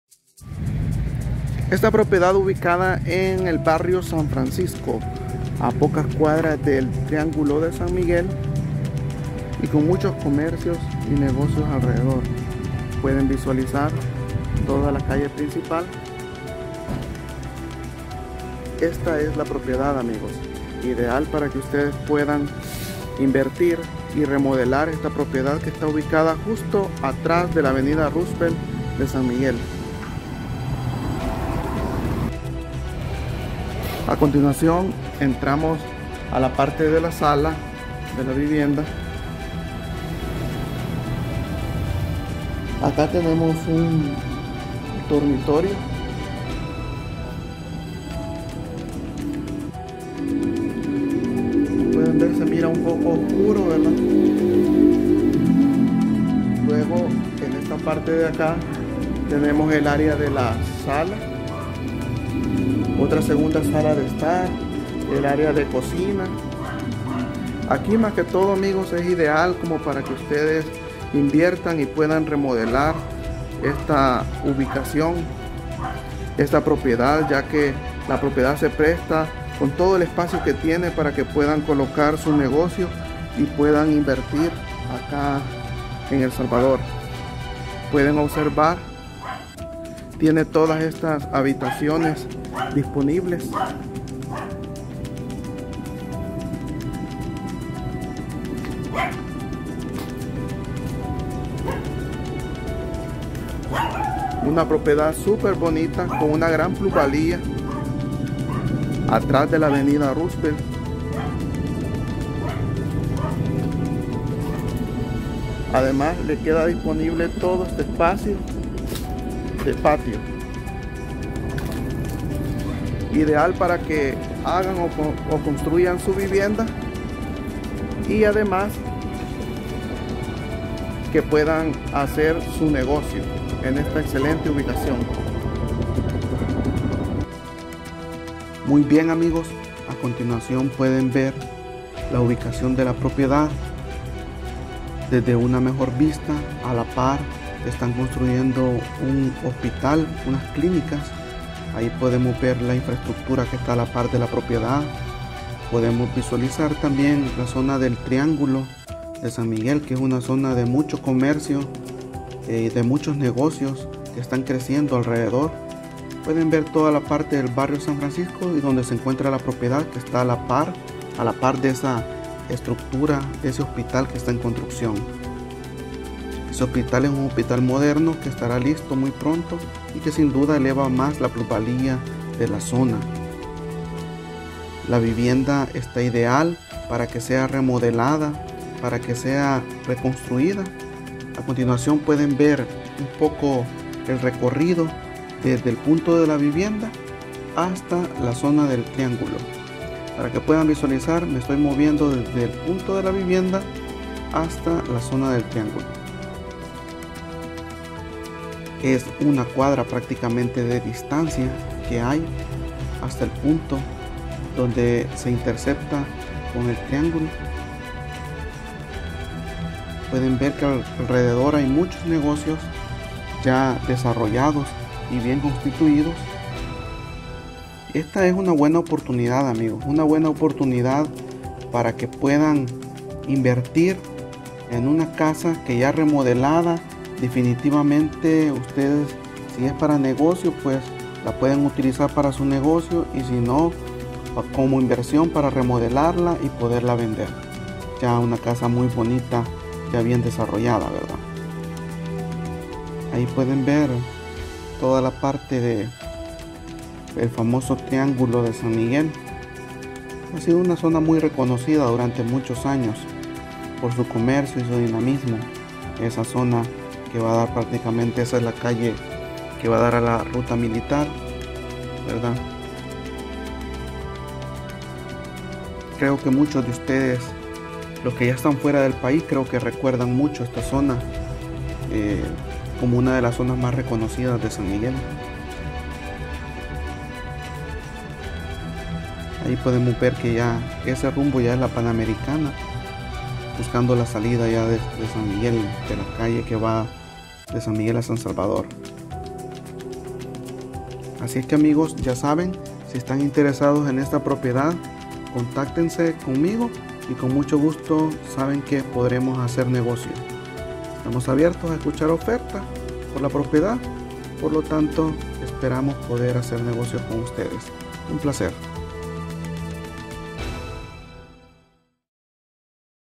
SE VENDE CASA A REMODELAR EN BARRIO SAN FRANCISCO SAN MIGUEL CONTIGUO A AVENIDA ROOSEVELT
Gran oportunidad de inversion para remodelar de esta vivienda ubicada en Barrio San Francisco contiguo a la Avenida Roosevelt a 50 metros del Triangulo de San Miguel.

Precio: $ 220,000.00 Negociable
Tamaño: 425.90 metros cuadrados